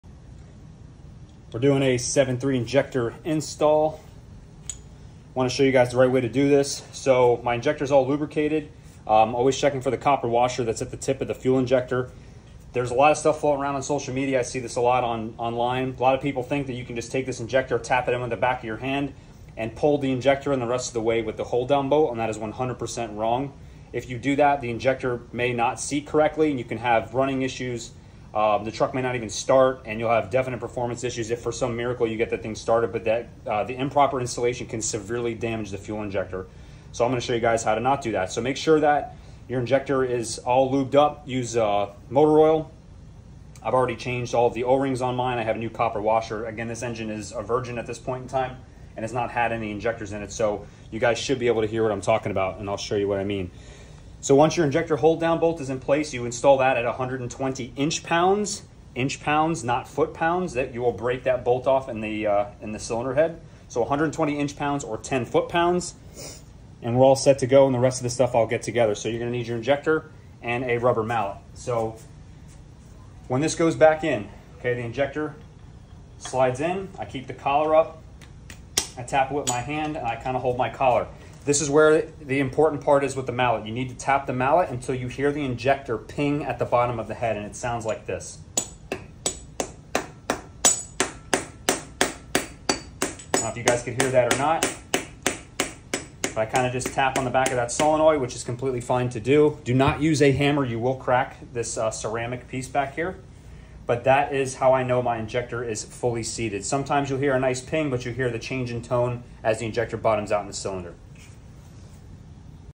7.3L Powerstroke injector install the RIGHT way
Install these properly, don’t let an improper technique be the demise of your engine!